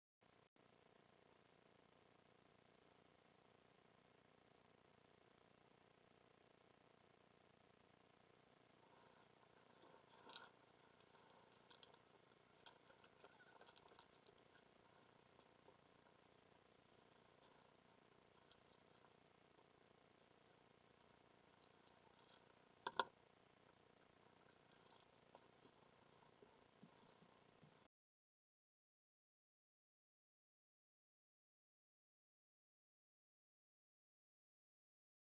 VosV 2014 04 20 15 31
Op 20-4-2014 om 20:15 uur vertrekt Vos V uit de nestkamer. Een paar minuten later vertrekt zij pas van de burcht.